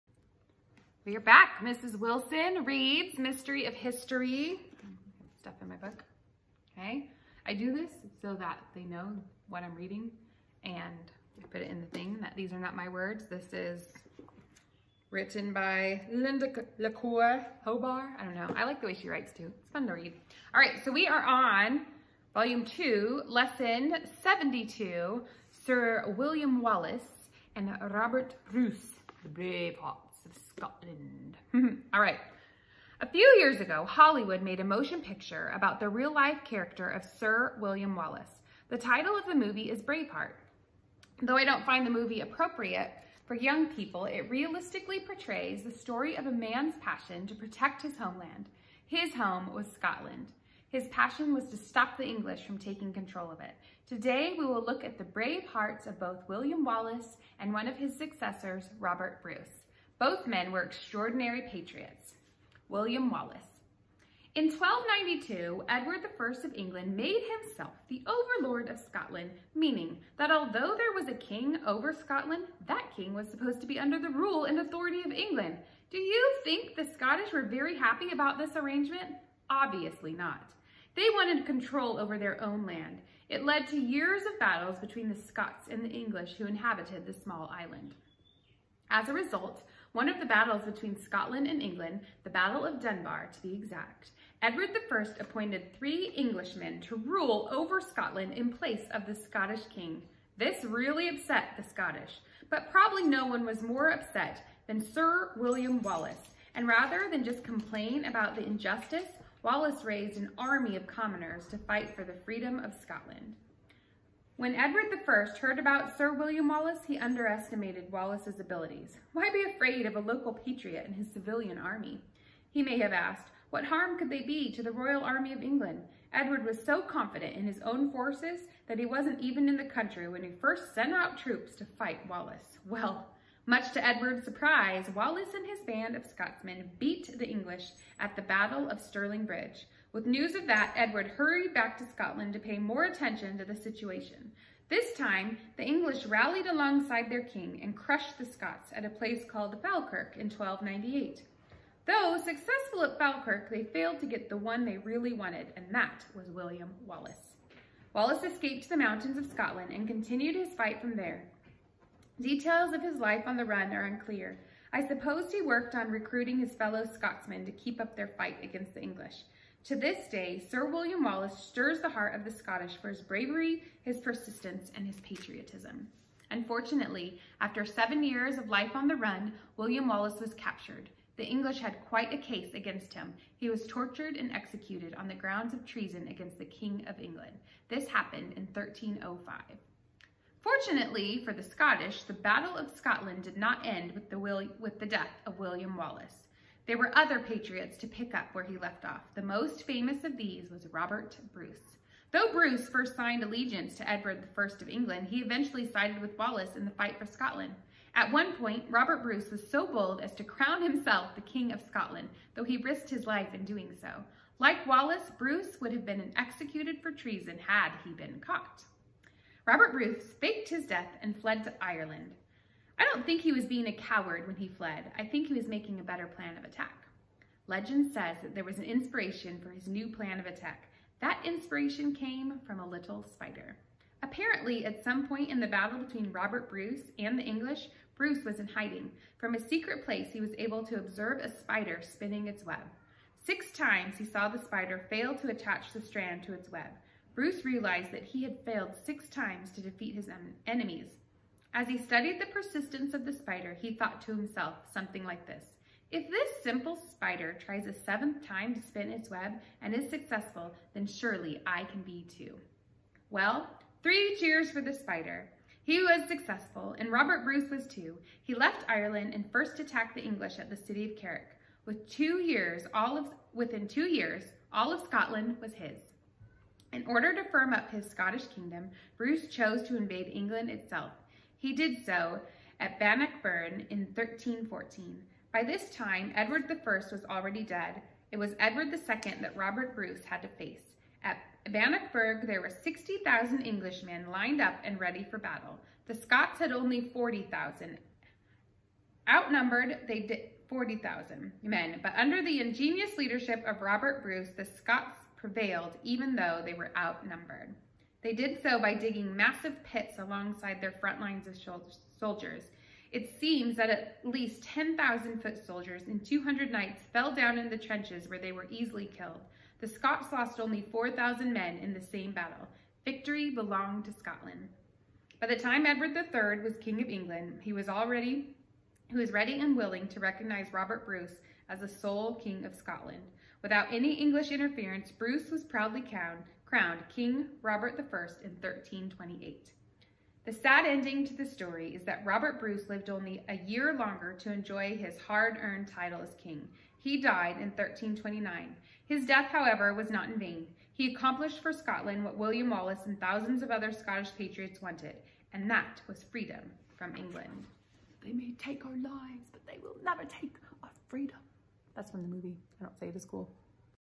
Mrs. Wilson reads Mystery of History vol2 lesson 72 Sir William Wallace
Sir William Wallace and Robert Bruce, “Bravehearts” of Scotland